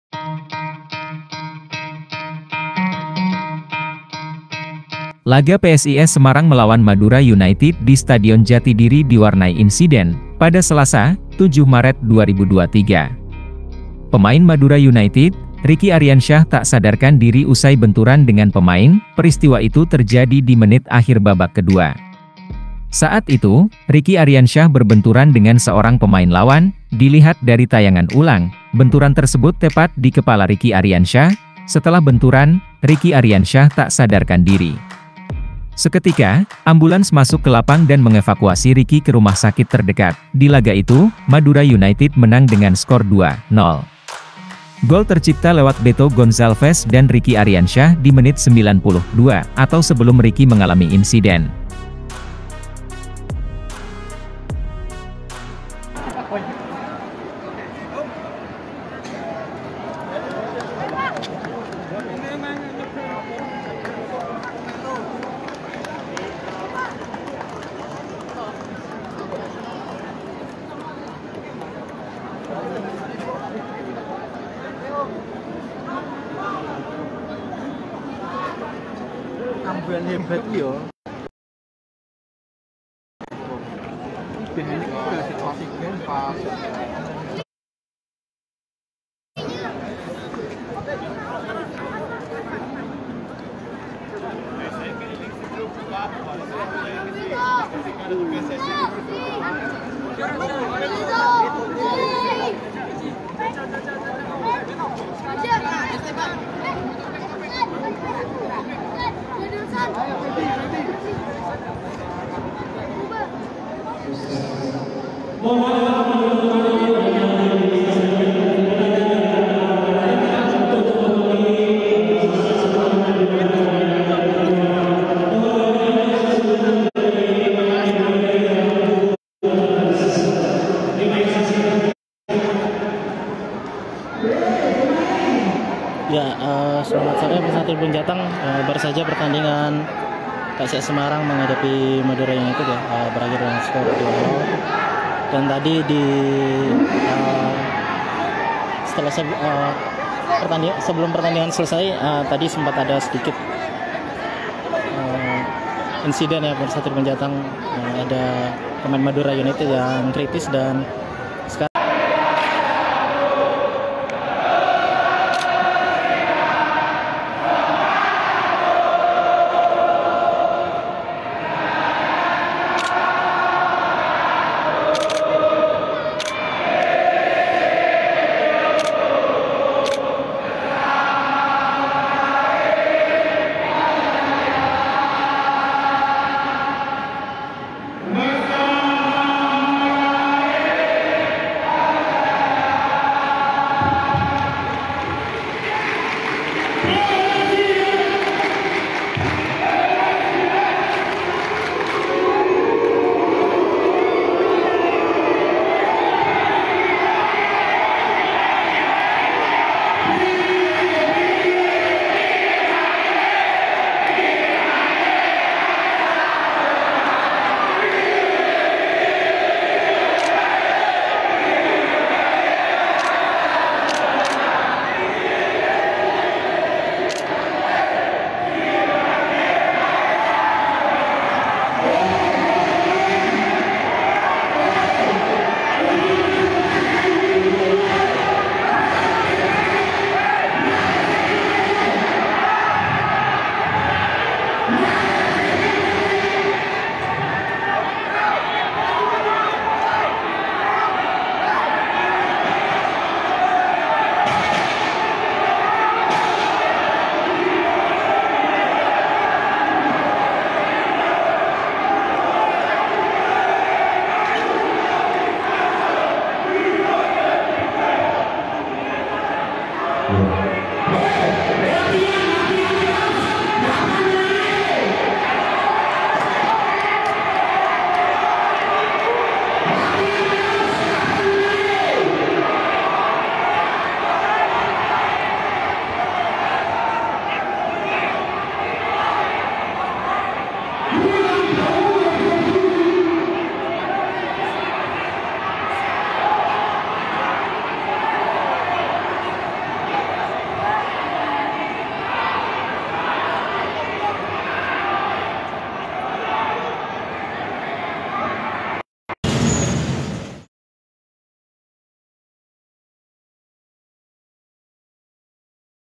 PEMAIN MADURA UNITED RICKY ARIANSYAH TERKAPAR, BENTURAN DI KEPALA, SUASANA USAI INSIDEN MENGERIKAN
TRIBUNJABARVIDEO- Laga PSIS Semarang melawan Madura United di Stadion Jatidiri diwarnai insiden, pada Selasa (7/3/2023). Pemain Madura United, Ricky Ariansyah tak sadarkan diri usai benturan dengan pemain.

Peristiwa itu terjadi di menit akhir babak kedua. Saat itu, Ricky Ariansyah berbenturan dengan seorang pemain lawan.

Dilihat dari tayangan ulang, benturan tersebut tepat di kepala Ricky Ariansyah.

Setelah benturan, Ricky Ariansyah tak sadarkan diri. Seketika, ambulans masuk ke lapang dan mengevakuasi Ricky ke rumah sakit terdekat.

Di laga itu, Madura United menang dengan skor 2-0. Gol tercipta lewat Beto Gonzalves dan Ricky Ariansyah di menit 90+2 atau sebelum Ricky mengalami insiden.